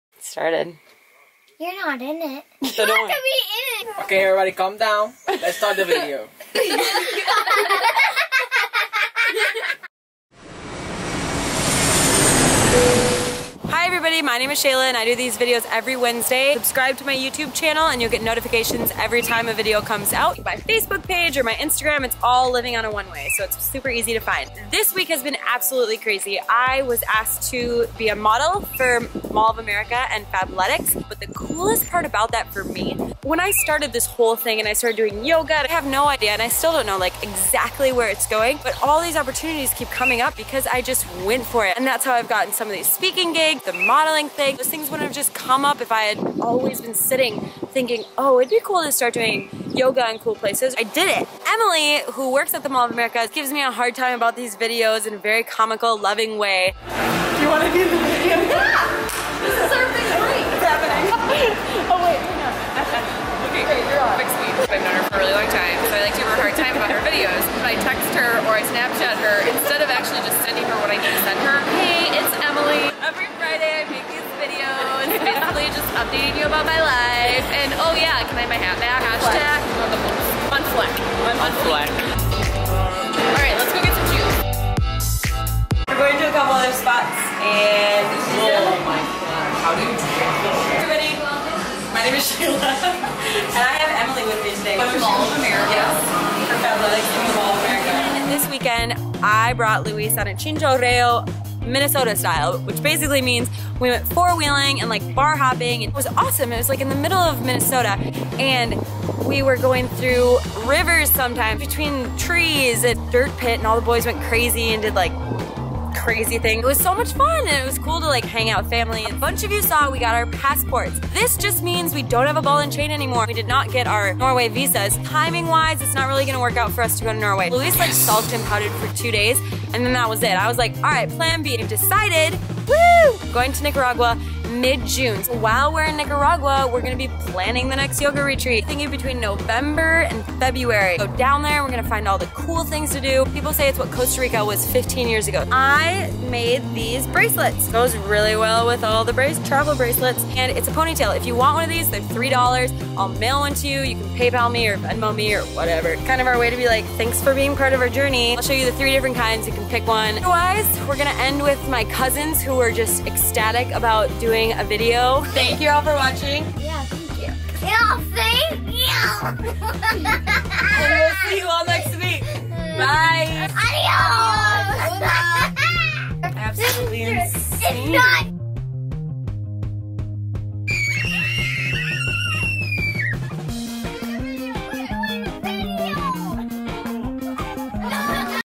The opportunities keep coming!
Where to Next?!
We had a busy week! Mall of America, Fabletics, 4 wheeling in Minnesota and new plans!!

‪#‎heyshayla‬ ‪#‎LetTheAdventuresBegin‬